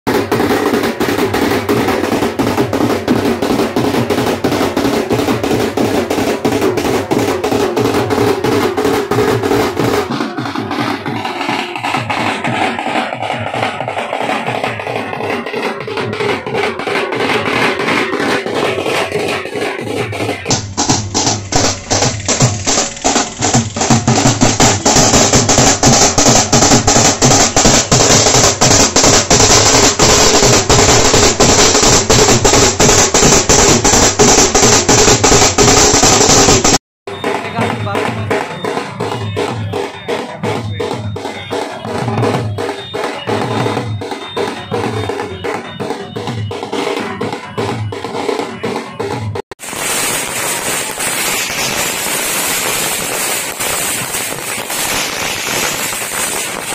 nikki:आतिशबाजी तथा मिठाई वितरण
टोक्यो ओलंपिक में पदक जीतने वाले भारतीय खिलाड़ियों के सम्मान में आज धनबाद जिला ओलंपिक संघ के के बैनर तले जिला खेल जगत के लोगों द्वारा झरना पाड़ा वॉलीबॉल ग्राउंड से रणधीर वर्मा चौक तक एक पैदल मार्च निकाली गई जो खिलाड़ियों द्वारा आतिशबाजी तथा मिठाई वितरण के साथ सफलता पूर्वक संपन्न हुई।